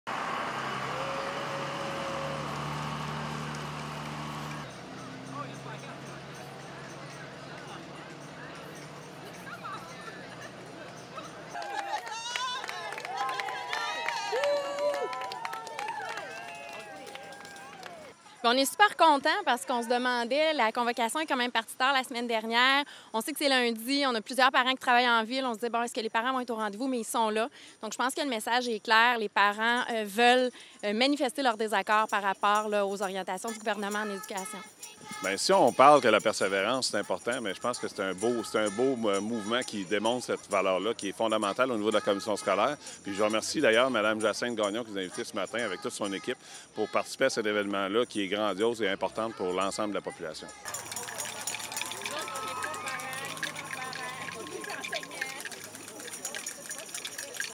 Chaîne humaine à St Laurent - 2015-11-02
Lundi matin, les parents démontrent leur appui aux enseignants à l'École St-Laurent du secteur Buckingham.